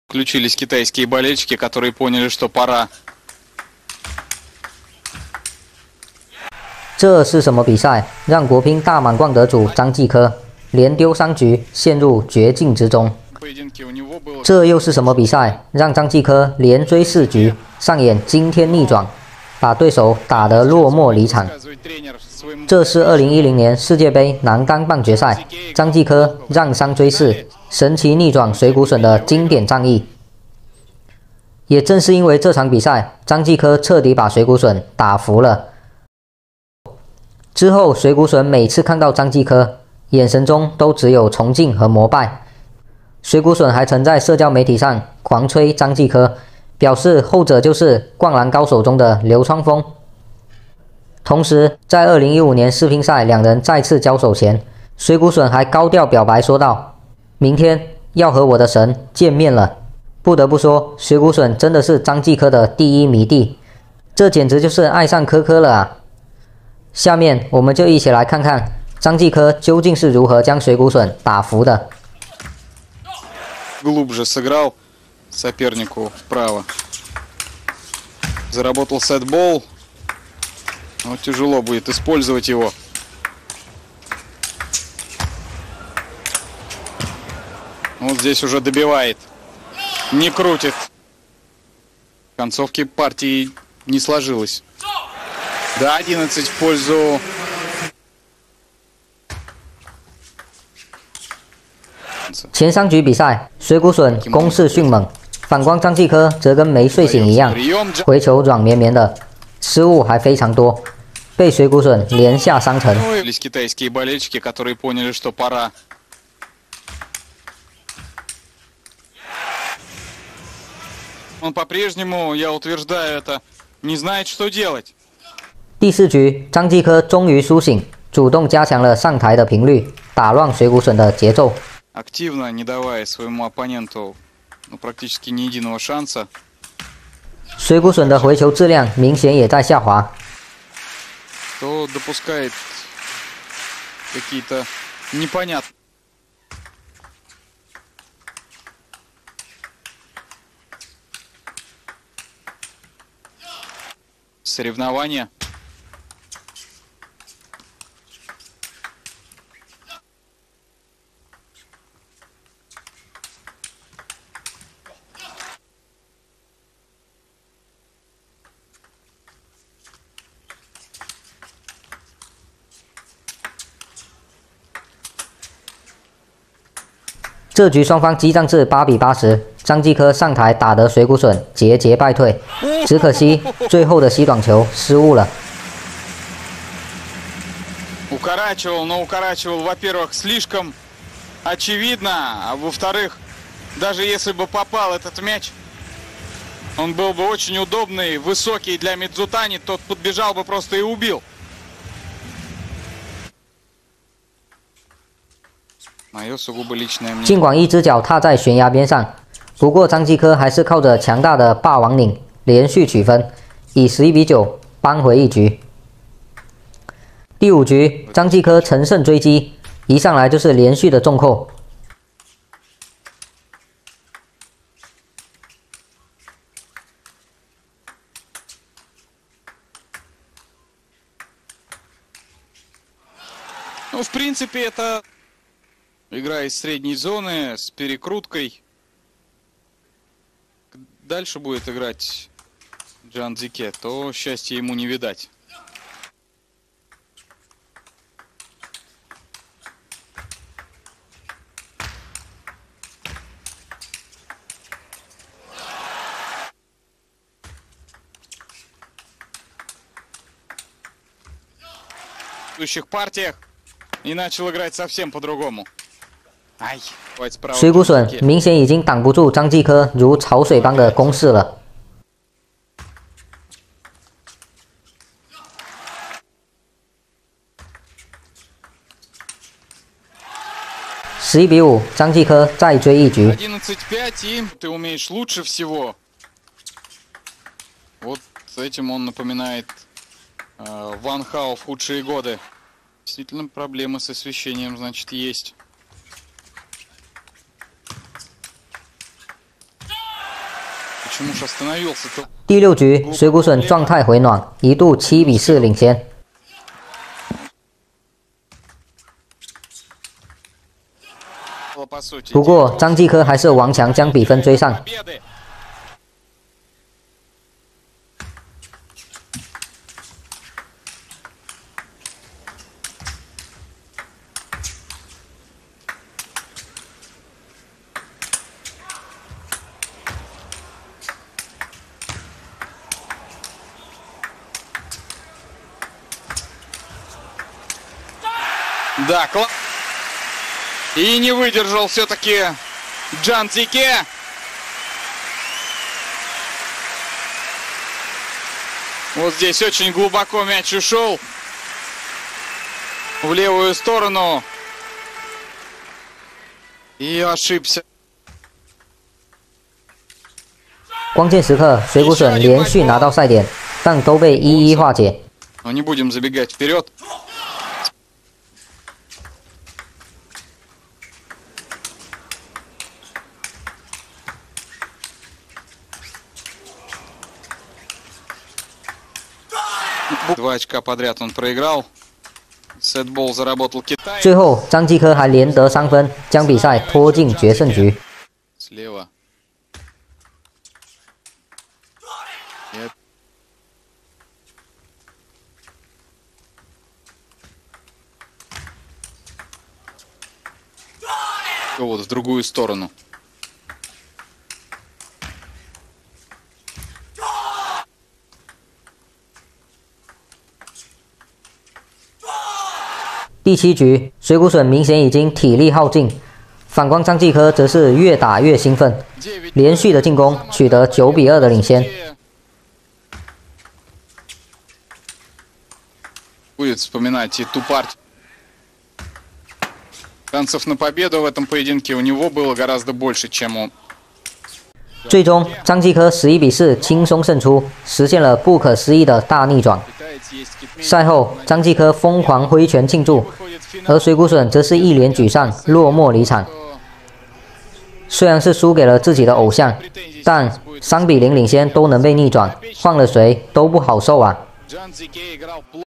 打不過你，我就愛上你！水谷隼3-0領先被大逆轉後：他就是我的神
本期為大家復盤，張繼科生涯最經典的一場逆轉。在2010年世界杯男單半決賽上，張繼科面對水谷隼，一度大比分0-3落後，但接下來卻連扳4局，上演驚天逆轉，徹底把水谷隼打服了。